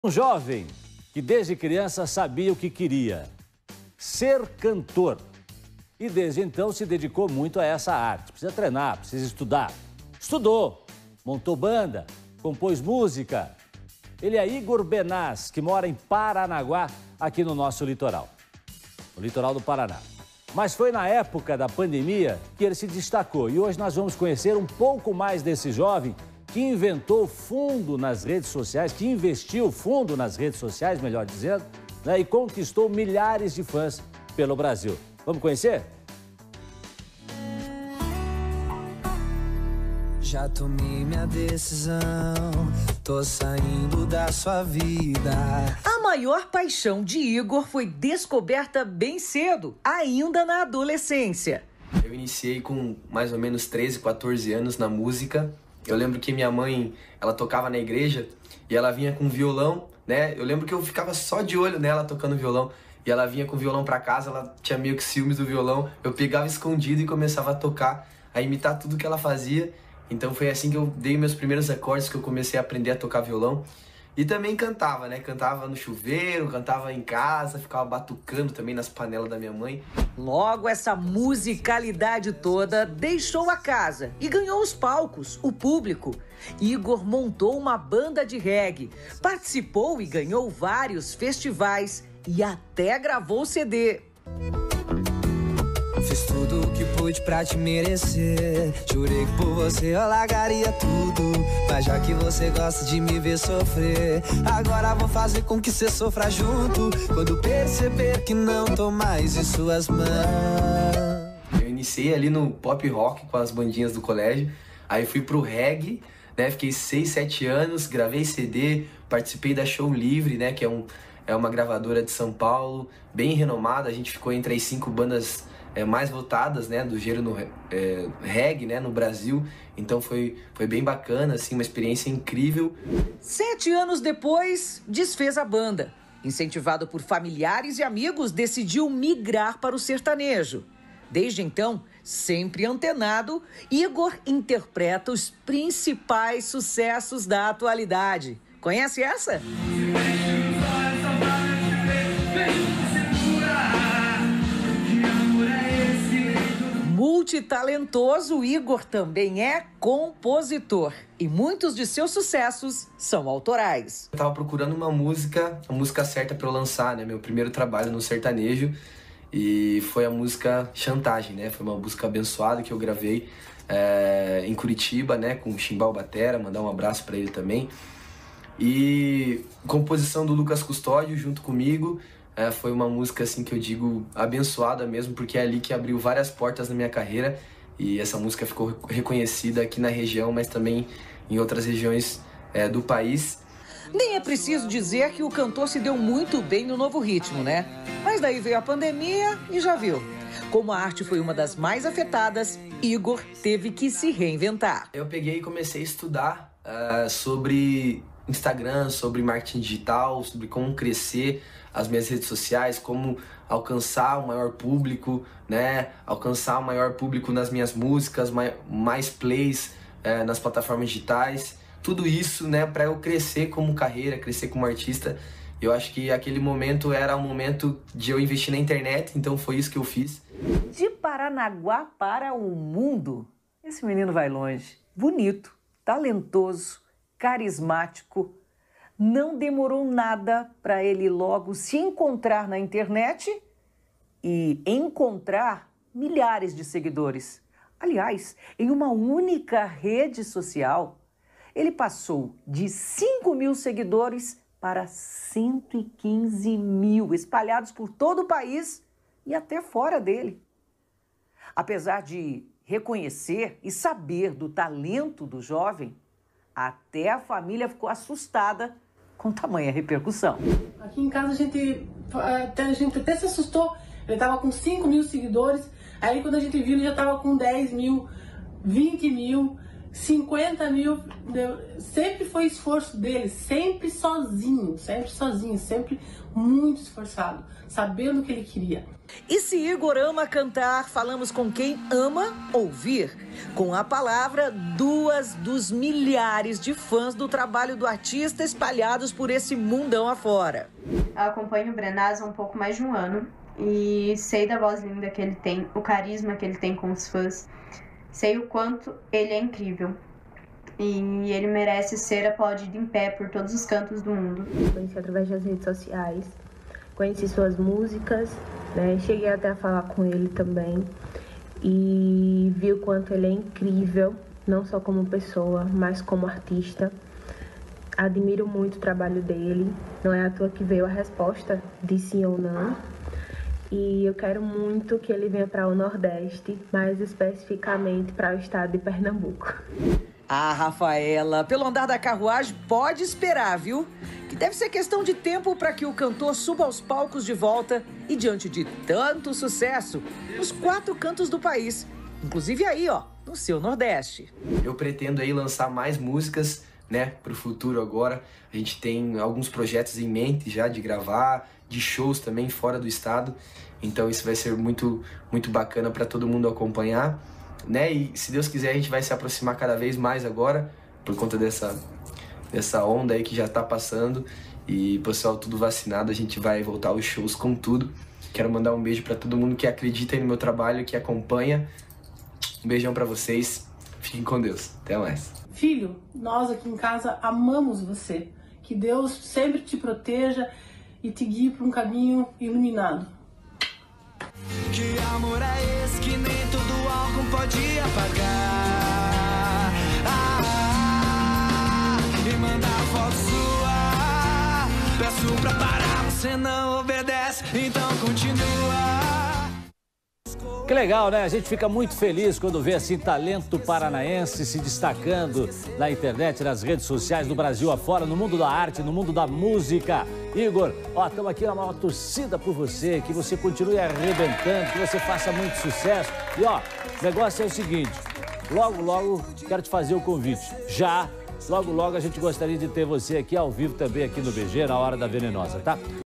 Cantor de Paranaguá conquista fãs de todo o país e se torna sucesso nas redes sociais
Um jovem que desde criança sabia o que queria: ser cantor e desde então, se dedicou muito à essa arte. Estudou, montou banda, compôs música. Ele é Igor Benaz, que mora em Paranaguá, no litoral do estado. Confira todos os detalhes na reportagem!